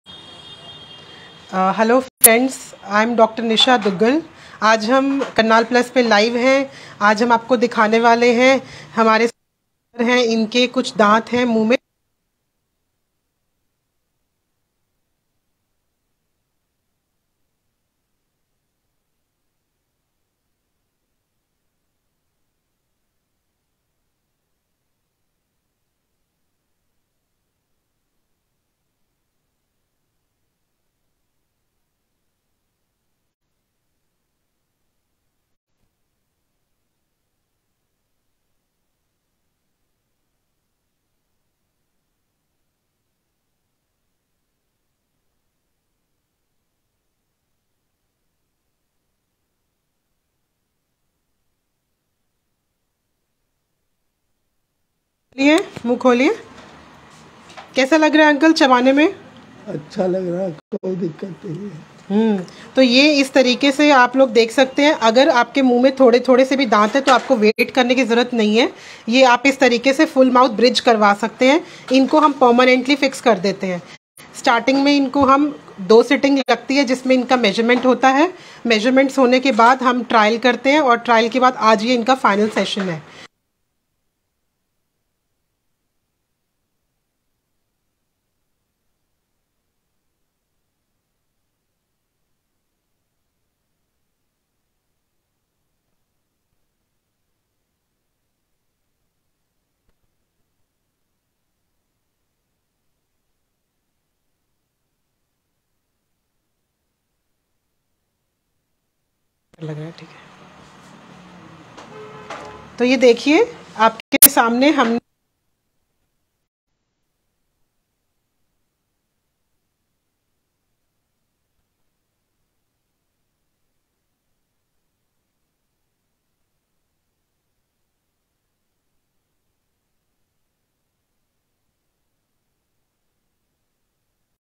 60 साल के बूढ़े अकल को बना दिया 18 साल का दांत लगाते ही Dr Nisha Duggal
For Appointment with Dr Nisha Duggal